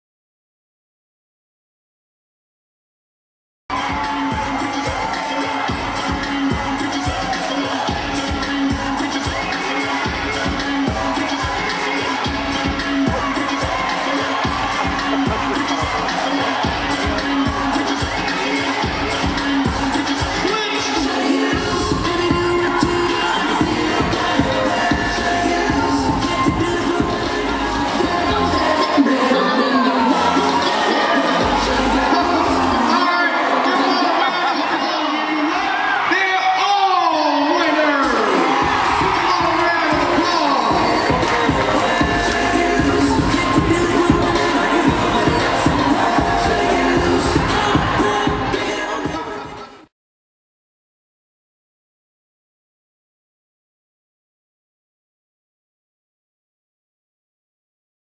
UniverSoul Circus - *Kids Dance* 04-05-08 8;00 show
Famous kids dance contest, universoul circus style!!